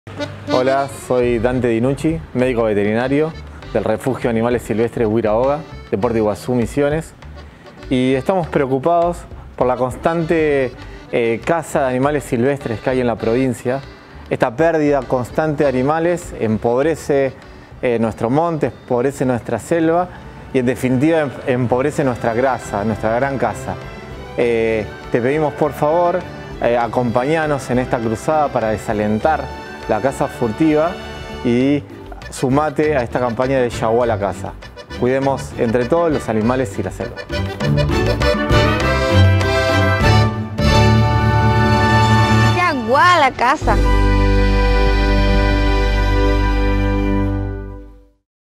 Yaguá la caza | Frenemos la caza de animales silvestres
⚠️ La pérdida de animales silvestres empobrece nuestra gran casa.
🟢 Aún estamos a tiempo de parar 🤚🏽 con la cacería, ¡Yaguá la Caza!
¿Te sumás 🫵🏼? Subí tu video 🤳🏽 desalentando la cacería con el hashtag Yaguálacaza y etiquetanos ⬆️. Digamos 🗣️ basta a la cacería.
⏩¡Cuidemos el monte y los animales!⏪